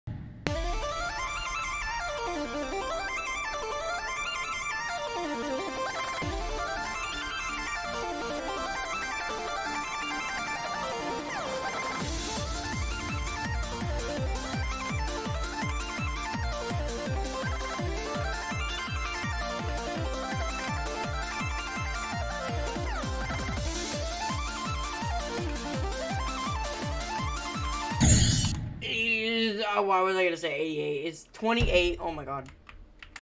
Progress 1 | SuperSonic 28%
Yep, I'm grinding supersonic as my next demon level and on 9/2/22 I'll be uploading 3 progress videos because I got 28%, 33%, and 37% in 40 attempts, absolutely crazy.

This is gonna be a great journey...